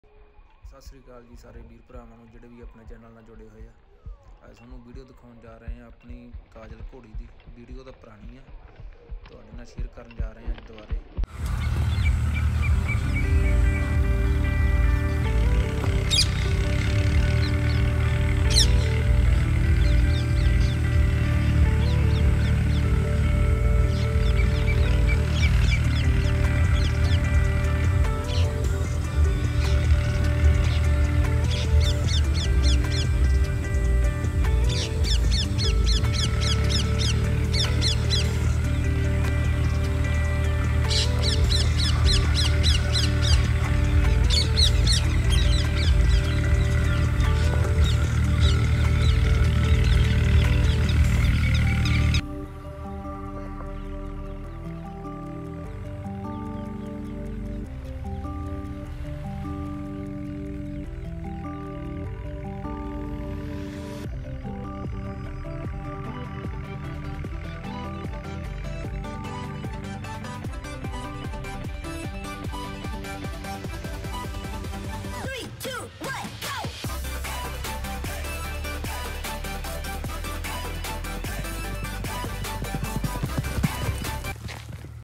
ਘੋੜੀ ਤੇ ਬੈਠ ਕੇ ਵੀਡੀਓ ਬਣਾਈ | ਘੋੜੀ ਦੀ ਰੇਵੀਆ ਚਾਲ ਤੇ ਸਪੀਡ ਦੇਖੋ
Kajol ghodi di rewia chal te speed check kro @Shokeenpunjabiharsangeet @horseofkutchgujarat9416 @shonkisardar7414